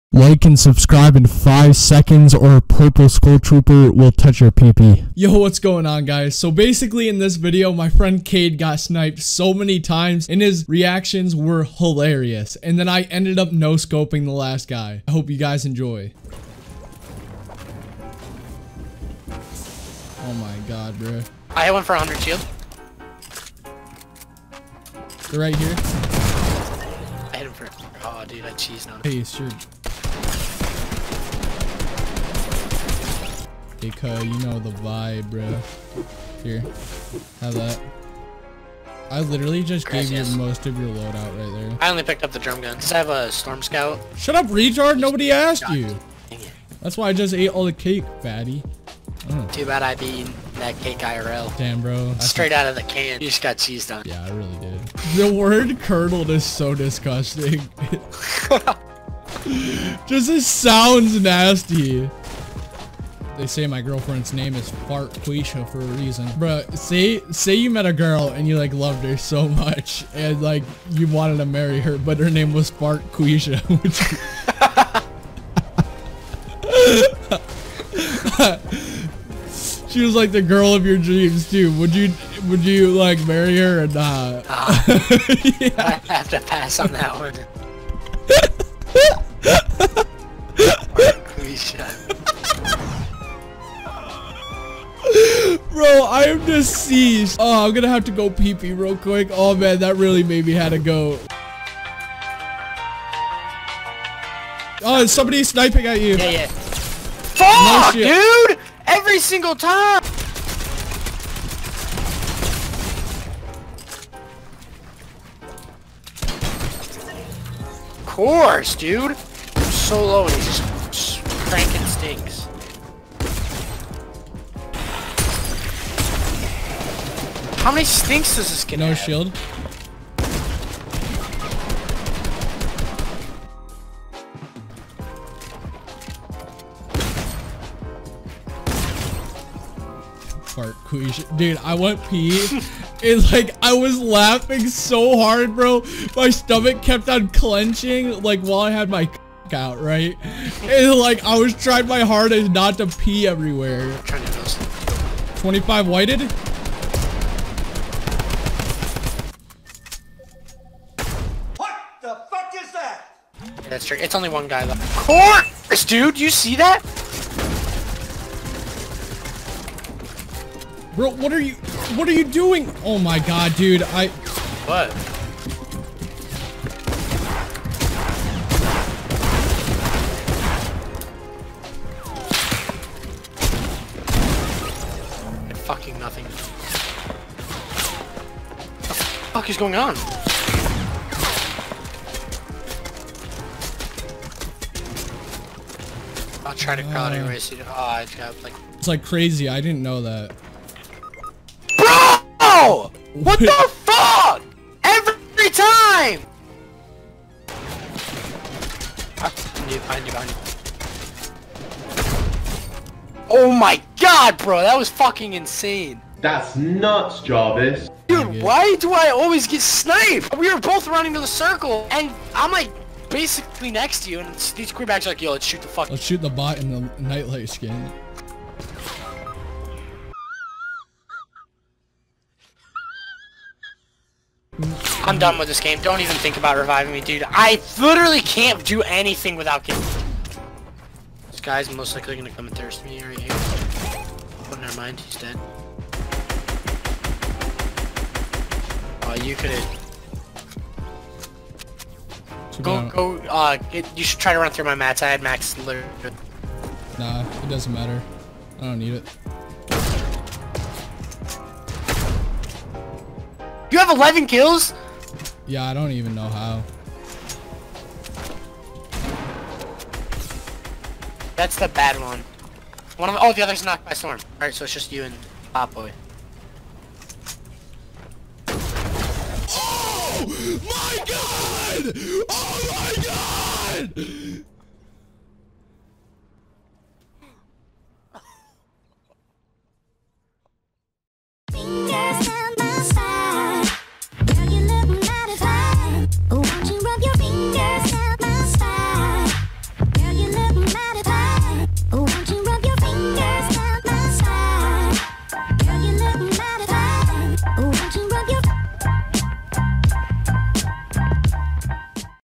My Duo kept getting snipped.. so i noscoped the last guy
So in this game my duo partner got sniped like ten times so i noscoped the last guy, if you liked the video make sure to sub :)

~ Nerdperior ♡